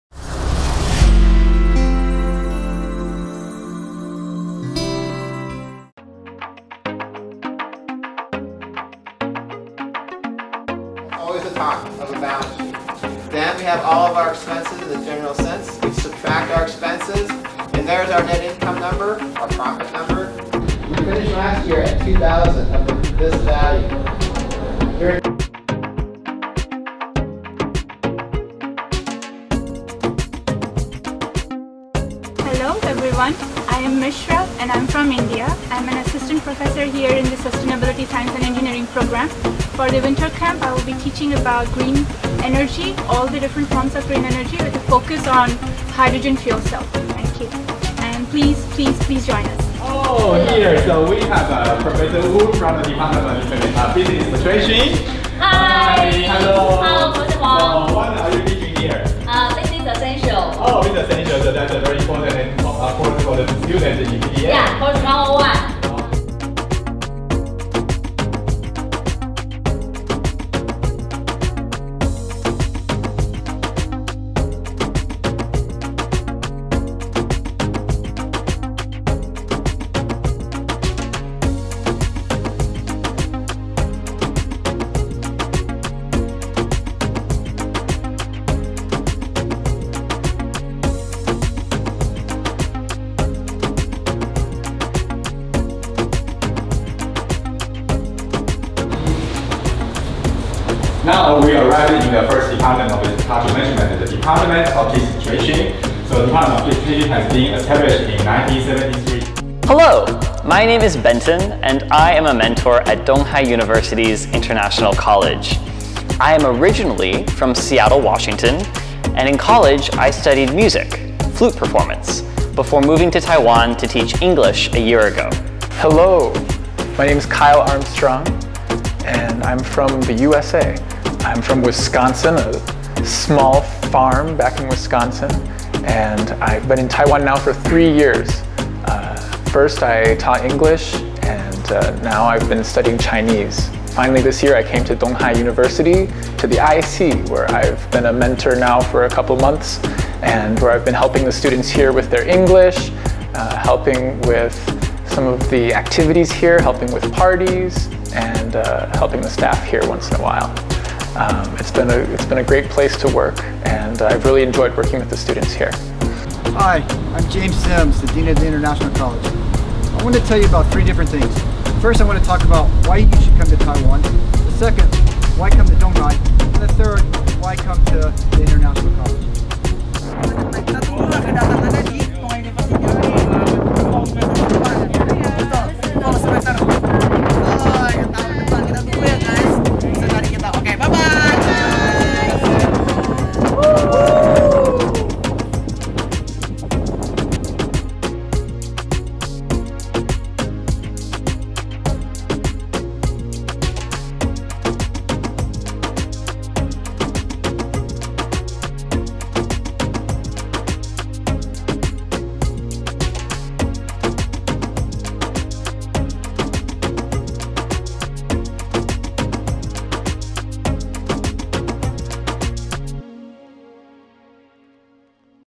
2021 THU Winter Camp- FACULTY & COURSES (18-22 Jan.)
general regulations
Invitee: 1st year or above Indonesian high school students (high school teachers are also welcome)
Course Date: 18-22, January 2021 (5 days)
Course Time: 13:10-17:00 (Jakarta time, 4 hours per day)
Course Fees: Free!
Certificate of Completion: For those who attended all online courses, a certificate of completion will be given
Estimated Number of Participants: Above 30 students for class to open